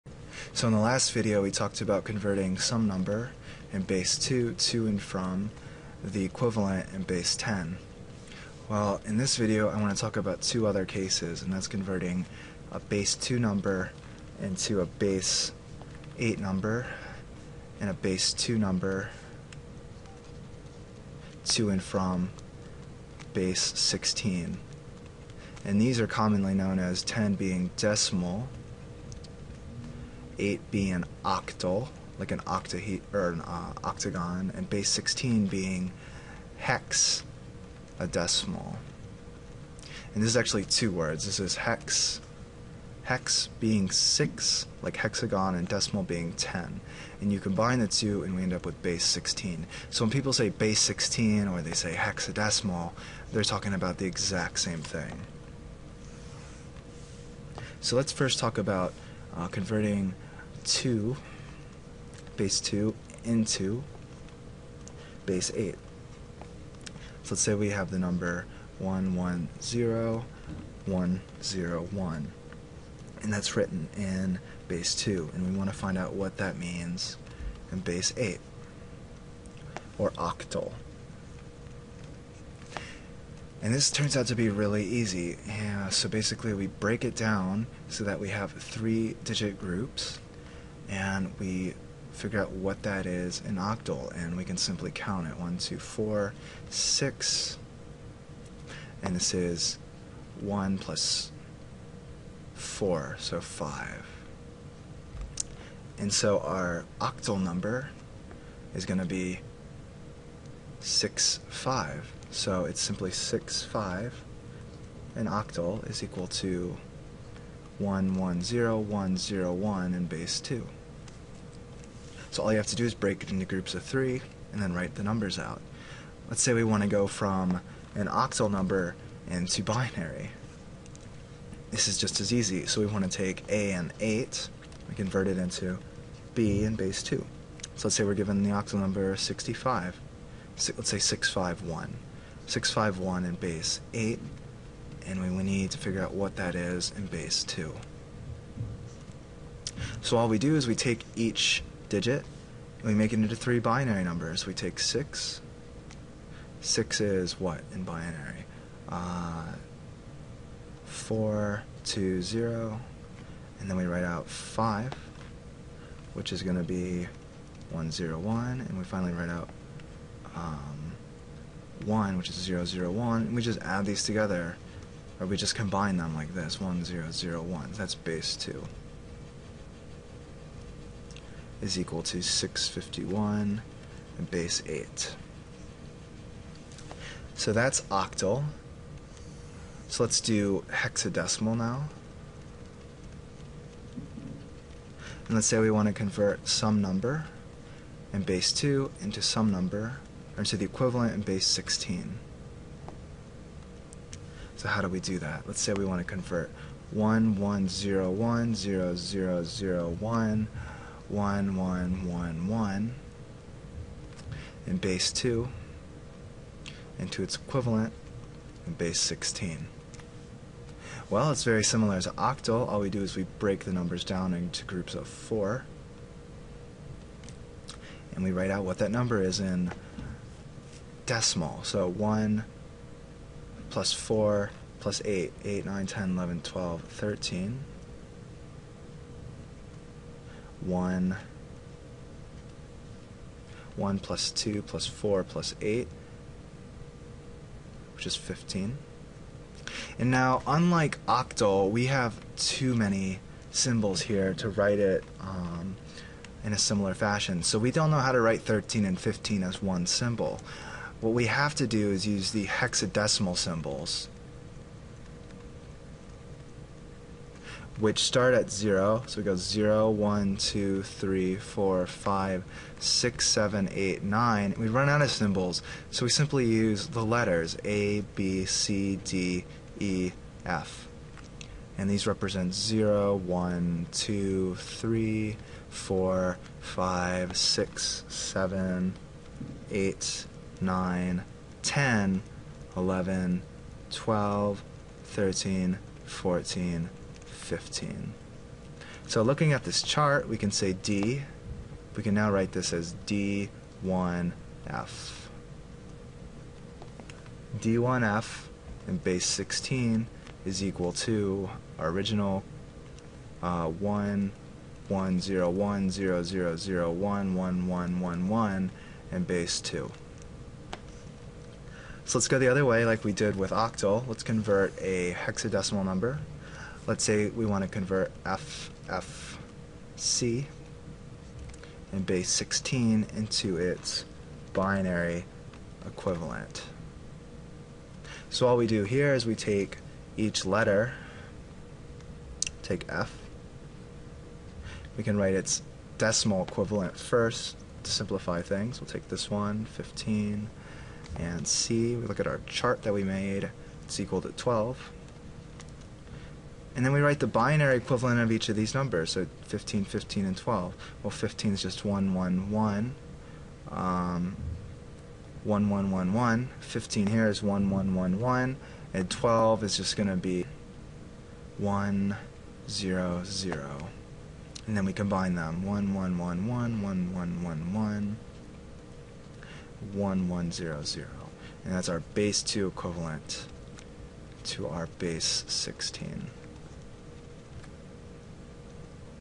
Introduction to Computer Engineering - Part 5 Binary to and from Octal and Hexadecimal
Convert binary to and from octal and hexadecimal.